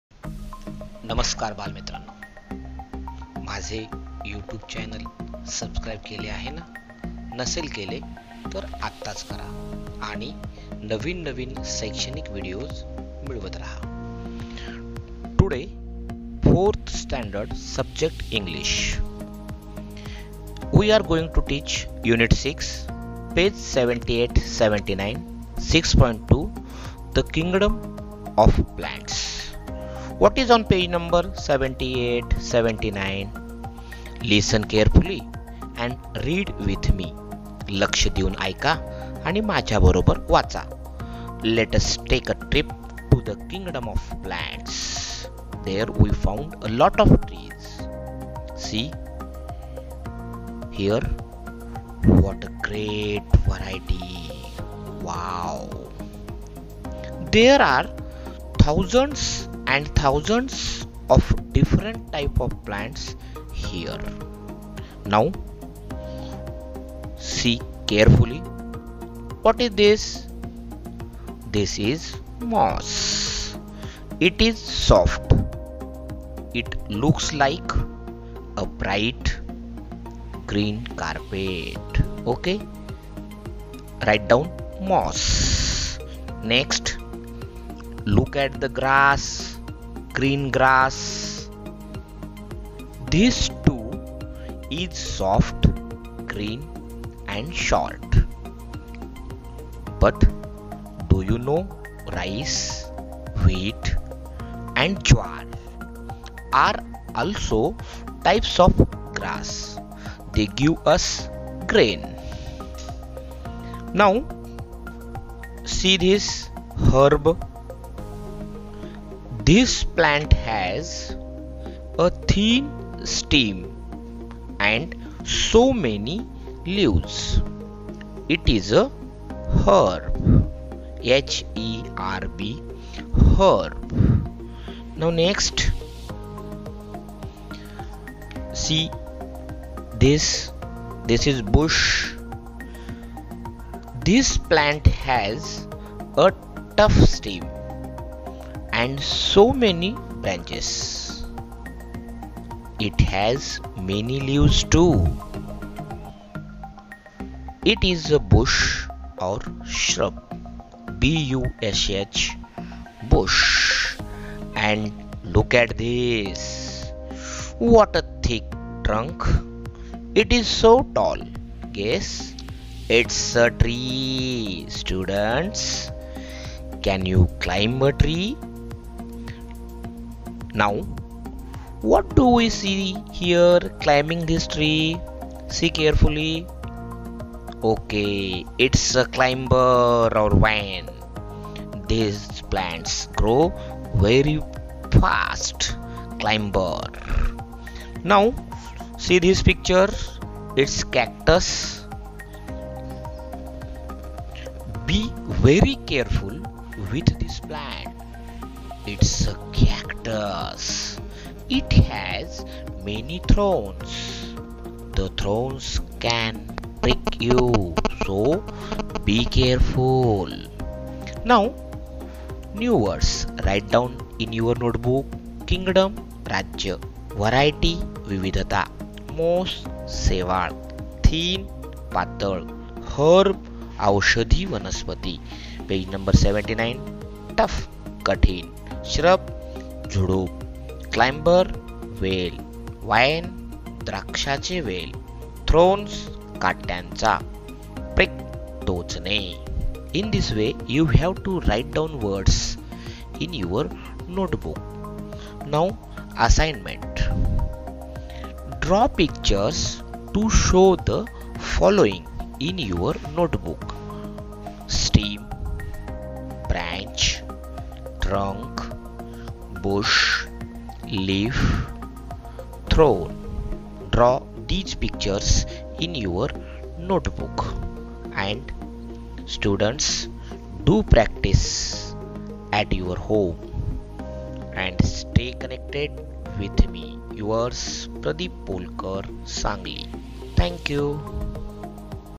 इयत्ता 4 थी विषय इंग्रजी Unit 6 Page No. 78,79 sub unit 6.2 The kingdom of plants
विद्यार्थ्यांना वाचन सराव व्हावा या उद्देशाने इयत्ता 4थी च्या इंग्रजी विषयाचा unit 6 मधील The kingdom of plants हा उपघटक बनवला असून हा पाठ आवडला तर like  आणि share करायला विसरू नका...
Std 4 th
Sub English
Unit 6
6.2 The kingdom of plants
Page No. 78, 79
प्रदिप पोलकर